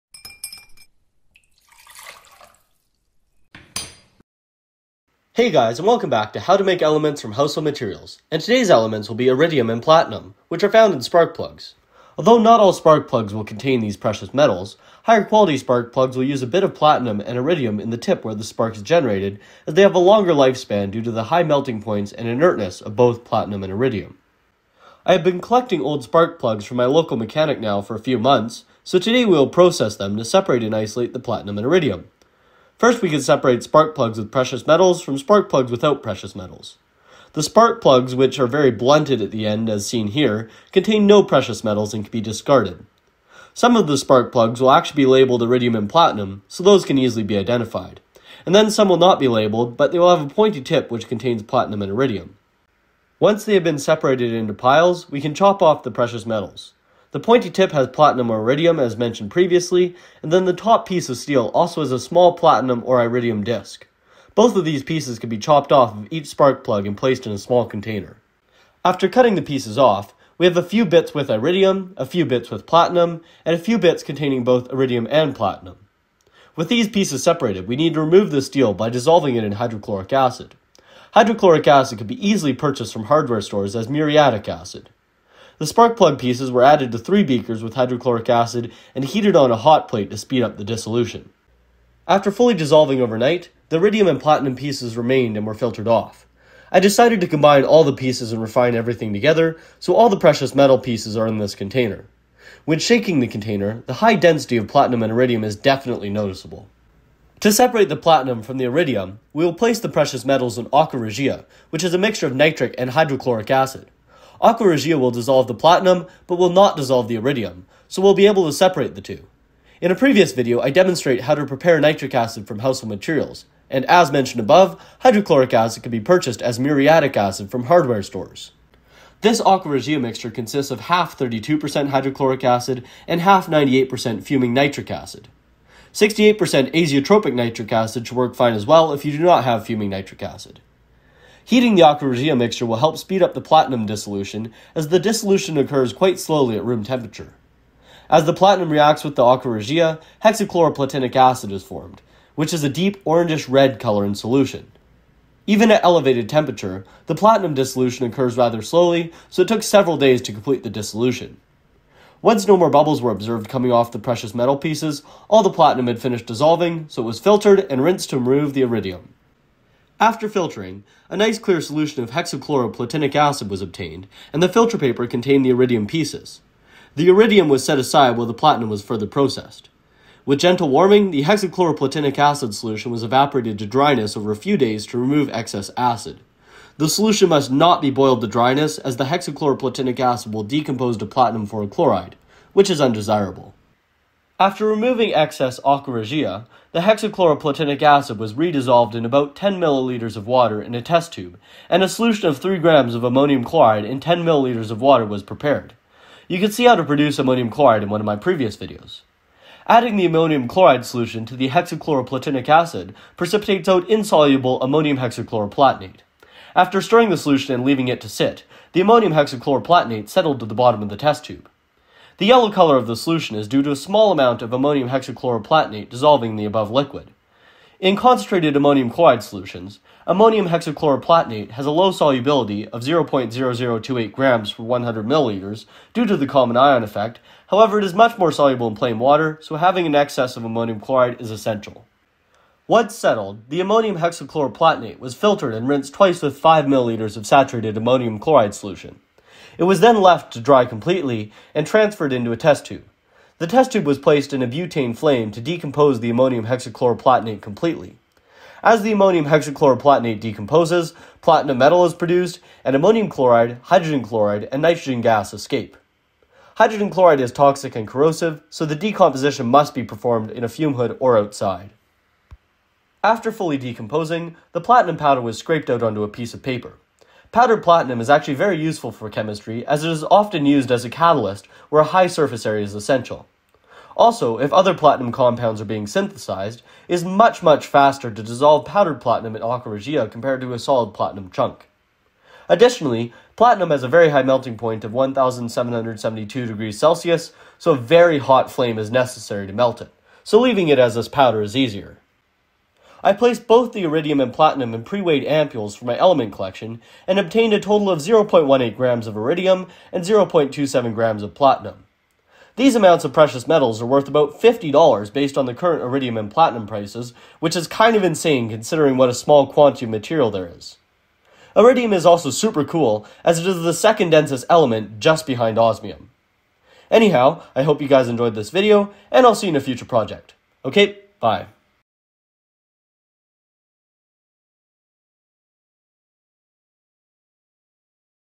Iridium and Platinum from Spark Plugs - Household Materials
In this video I demonstrate how to isolate iridium and platinum from old spark plugs that I collected from my local mechanic. We also get to explore a few fun platinum compounds in the process.